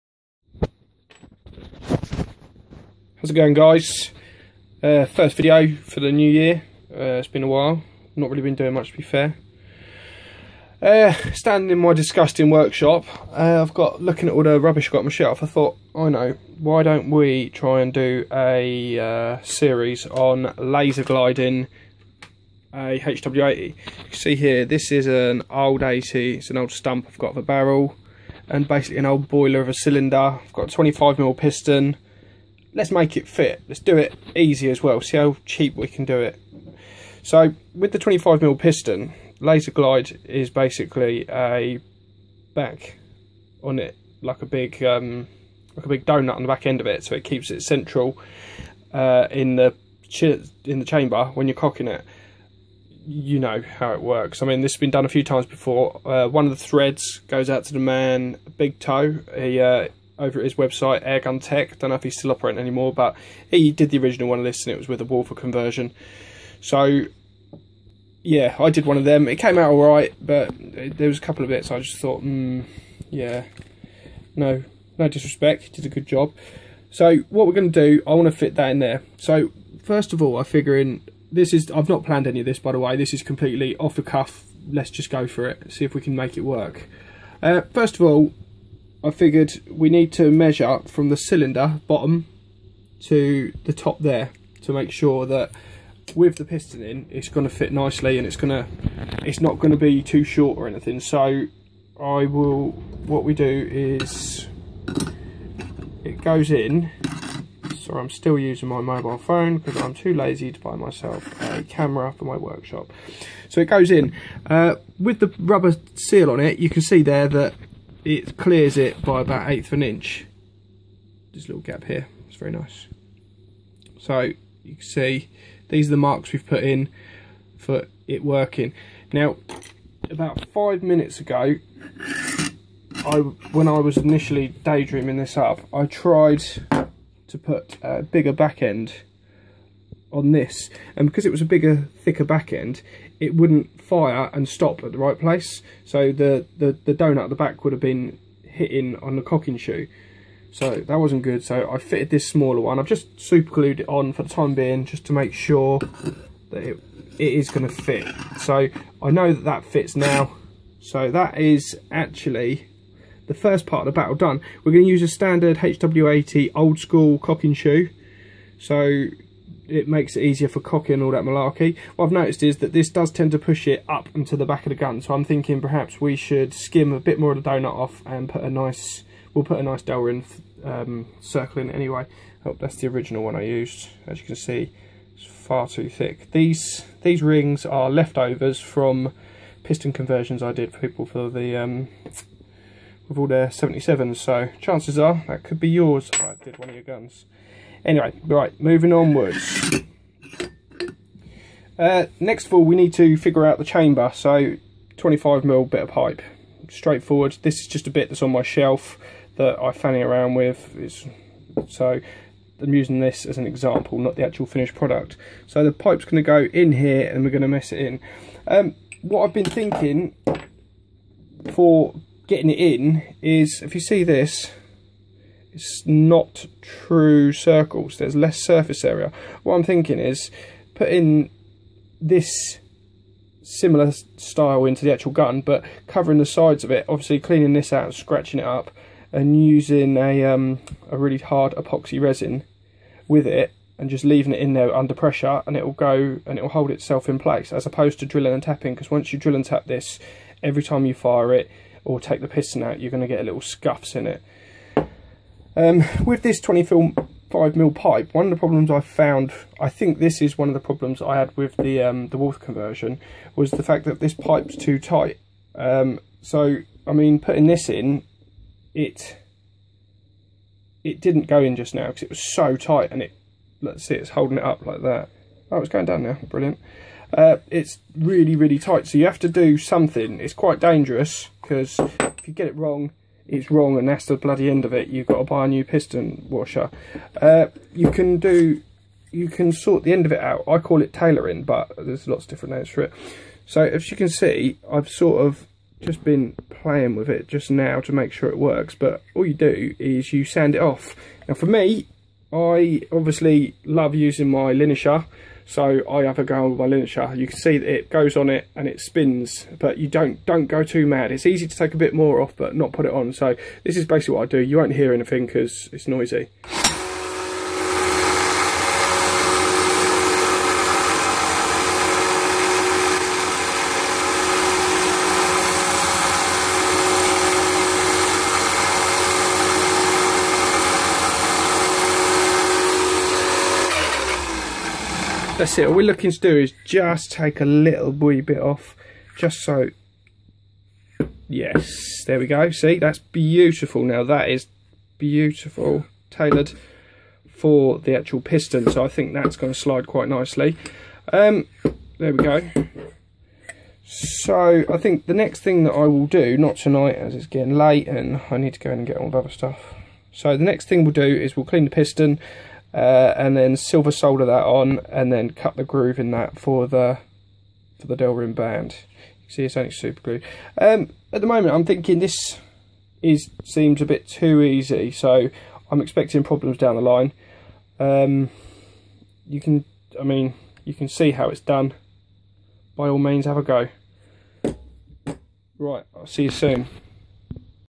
Weihrsuch Hw80 lasaglide buildalong
Ive been thinking about building a reduced diameter piston hw80 using no specalist parts just bits off the shelf for a while. I dont know if itll work or not but it seems doable.